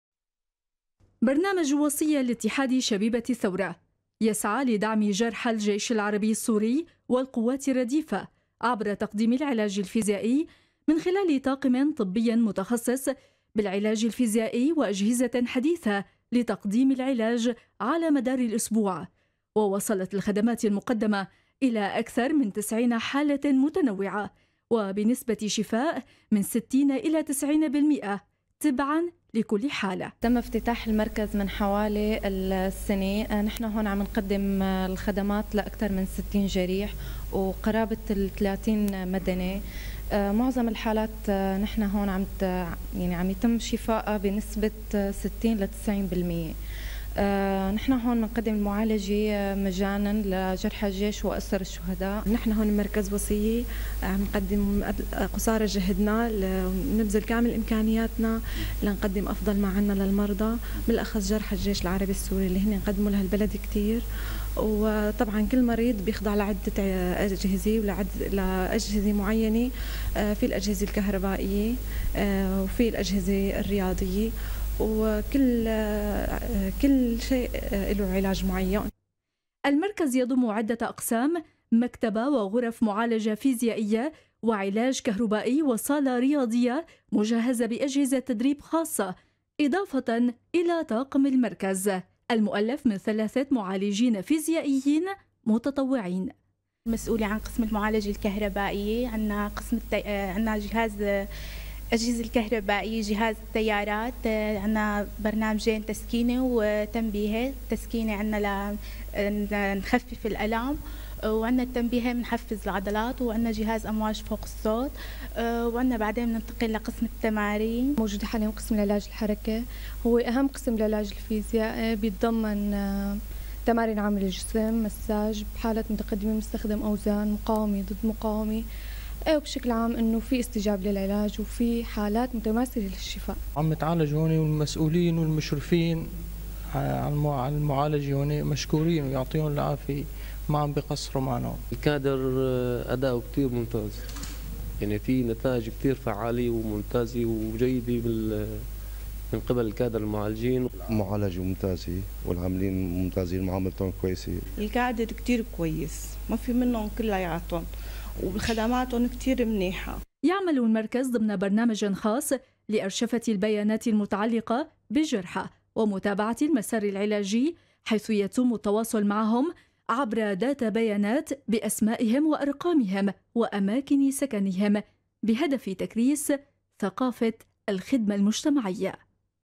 حماة | "وصية "مركز للعلاج الفيزيائي لجرحى الجيش العربي السوري في مدينة سلمية
تقرير علي شحود & زينب عاقل